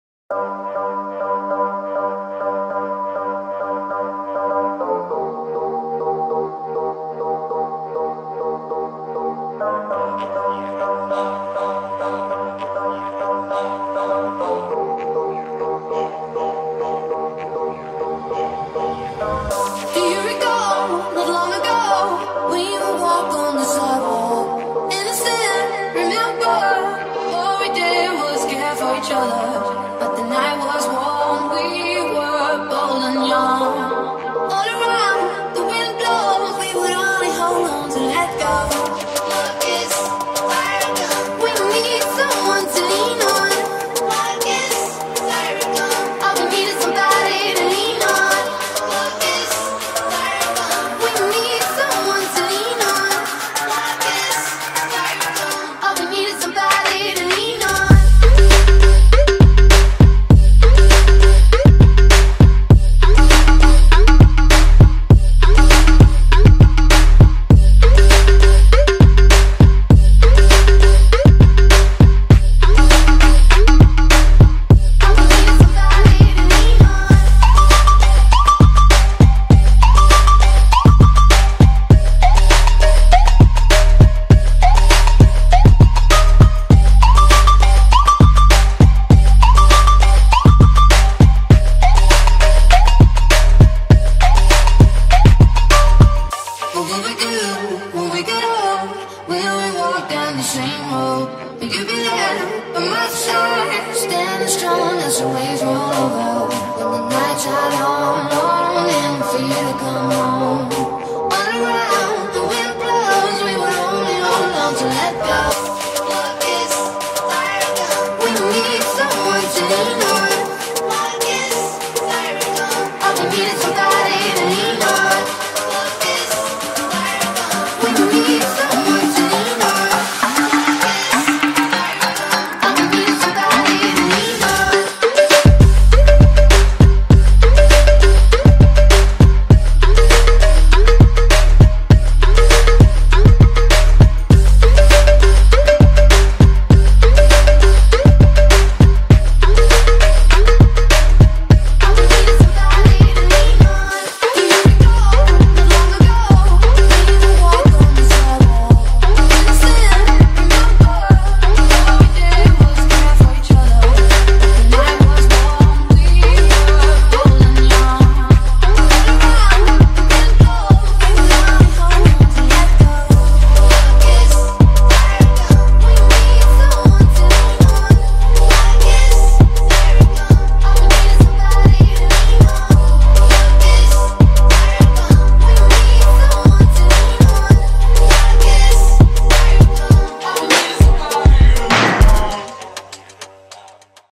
Anti-Racism Poster [Speedart] - "Society"
Hello Guys and Gals

today i decided to make an anti-racism inspirational poster it was because of one of my mates SkyyzGraphics he did a remake of KingTuts anti bullying poster this basically sparked an idea in my head.

*Stalk me HEHEHEHEEHE*

Skype: Seguranator2001

*Music Info*